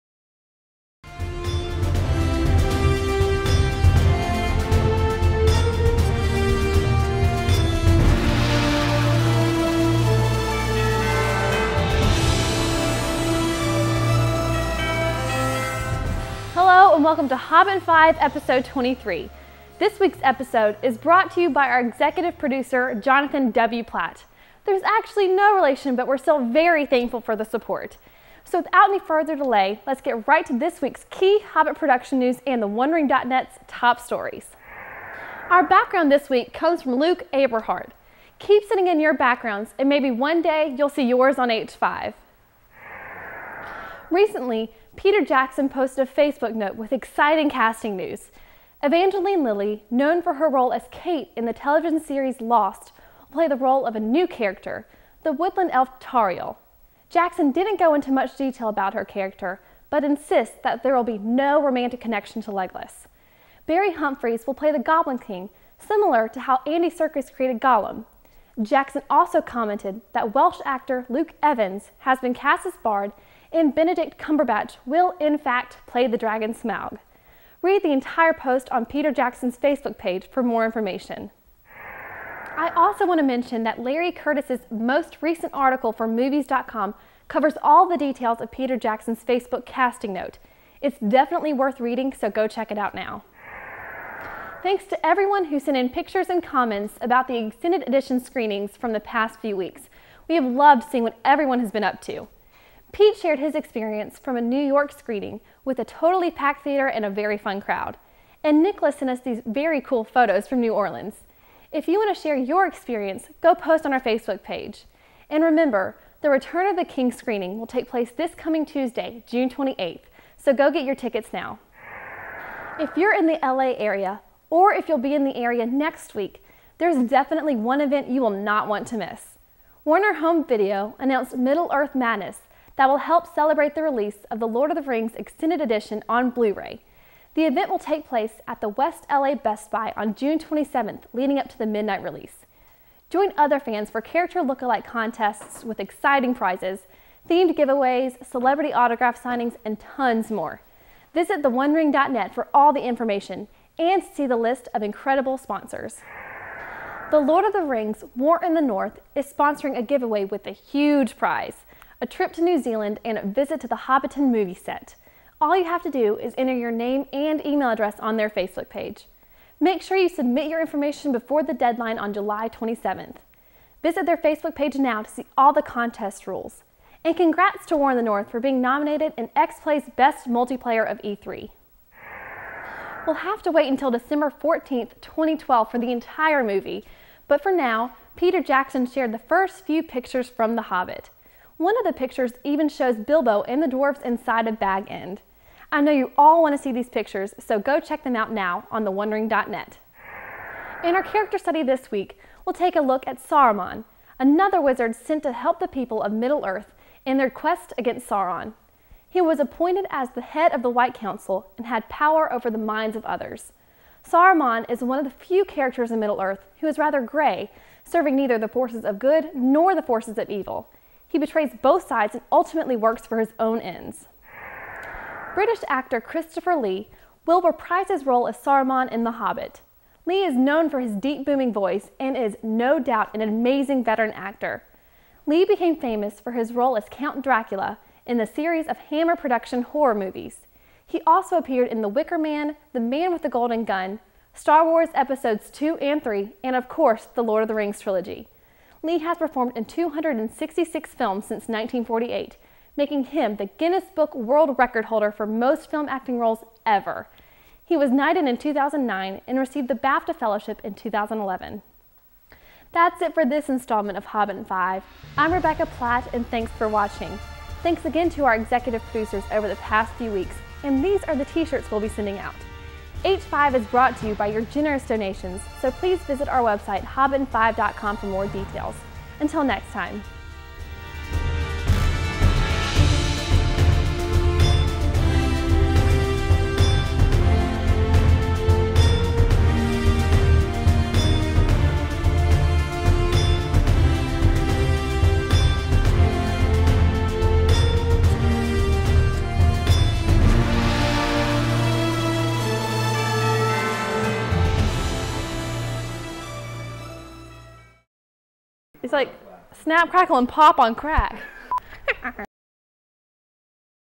Hobbit in 5 - Casting News, LA Release Party, New Pictures
On this episode of Hobbit in 5, we talk about Peter Jackson's newest Facebook note, share some viewer experiences, let you know of a Blu-Ray release party in LA, and give you a look at some new pictures from The Hobbit set! We also cover Saruman for our character study!

Thanks to our executive producer: Jonathan W. Platt

June 24th, 2011 - Episode 23

Rebekah has her own Facebook page now! Go check it out and "like" her!

Go like theOneRing.net on Facebook as well!